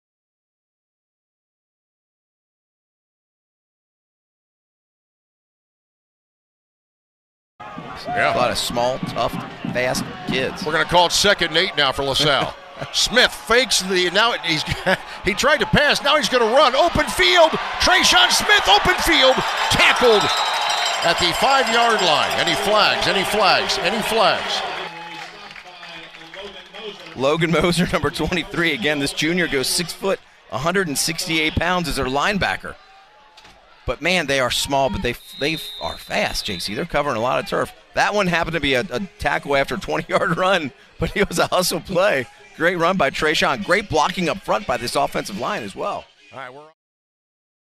TreSean Smith Run
QB TreSean Smith with the keeper for a nice gain against East Central.